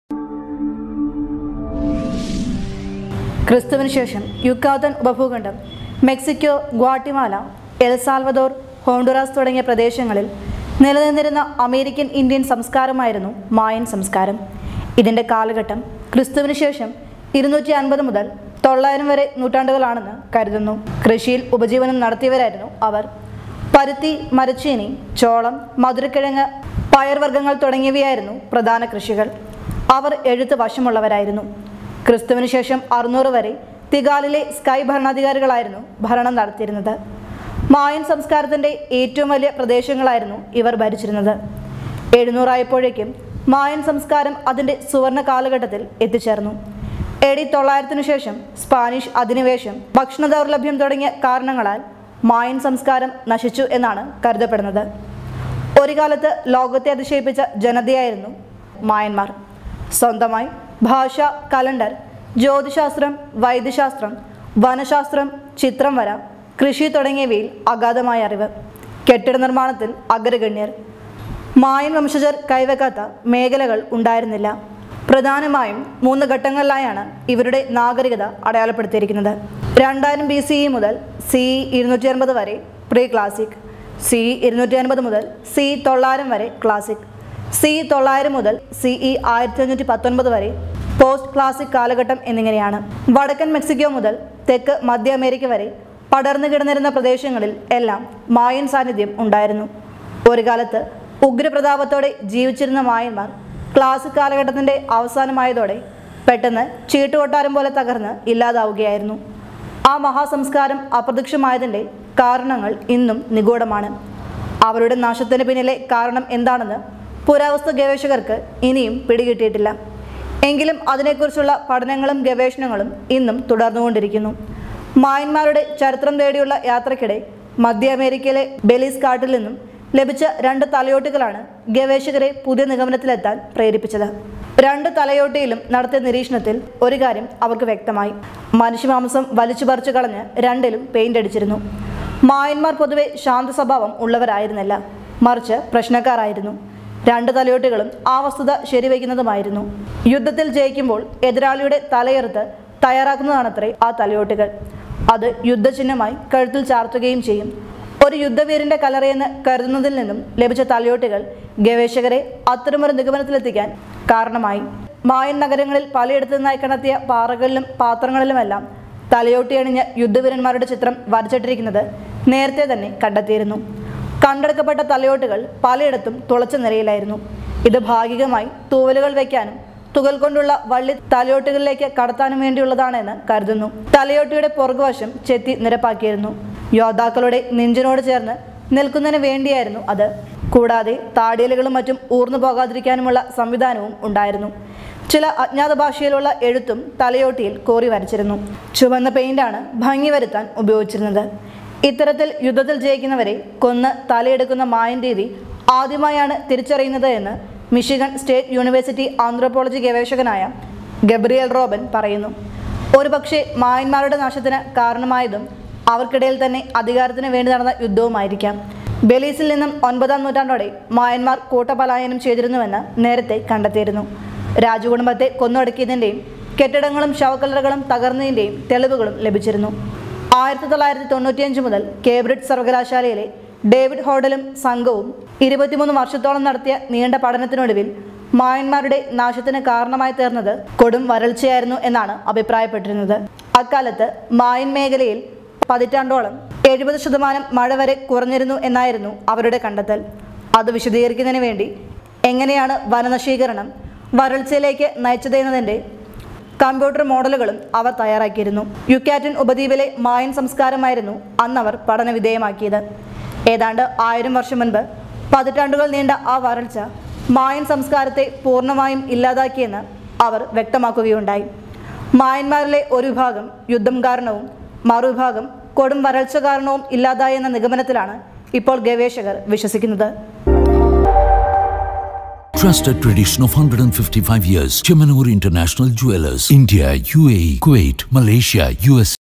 അറിയാം മായൻ സംസ്കാരത്തെക്കുറിച്ച് Anweshanam.com
ക്രിസ്തുവിന് ശേഷം യുക്കാത്തൻ ഉപഭൂഖണ്ഡം, മെക്സിക്കൊ, ഗ്വാട്ടിമാല, എൽ സാൽവദോർ, ഹോണ്ടുറാസ് തുടങ്ങിയ പ്രദേശങ്ങളിൽ നിലനിന്നിരുന്ന അമേരിക്കൻ-ഇൻഡ്യൻ സംസ്കാരമായിരുന്നു മായൻ സംസ്കാരം. ഇതിൻറെ കാലഘട്ടം ക്രിസ്തുവിന് ശേഷം 250 മുതൽ 900 വരെ നൂറ്റാണ്ടുകളാണെന്ന് കരുതുന്നു. കൃഷിയിൽ ഉപജീവനം നടത്തിയവരായിരുന്നു. പരുത്തി, മരച്ചീനി, ചോളം, മധുരക്കിഴങ്ങ്, പയറ് വർഗ്ഗങ്ങൾ തുടങ്ങിയവയായിരുന്നു പ്രധാന കൃഷികൾ. അവർ എഴുത്ത് വശമുള്ളവരായിരുന്നു. ക്രിസ്തുവിന് ശേഷം 600 വരെ തികാലിലെ സ്കൈ ഭരണാധികളായിരുന്നു ഭരണം നടത്തിയിരുന്നത്. മായൻ സംസ്കാരത്തിൻറെ ഏറ്റവും വലിയ പ്രദേശങ്ങളായിരുന്നു ഇവർ ഭരിച്ചിരുന്നത്. 700 ആയപ്പോഴേക്കും മായൻ സംസ്കാരം അതിൻറെ സുവർണ്ണകാലഘട്ടത്തിൽ എത്തിച്ചേർന്നു. എ.ഡി. 900 ന് ശേഷം സ്പാനിഷ് അധിനിവേശം, ഭക്ഷണ ദൗർലഭ്യം തുടങ്ങിയ കാരണങ്ങളാൽ മായൻ സംസ്കാരം നശിച്ചു എന്നാണ് കരുതപ്പെടുന്നത്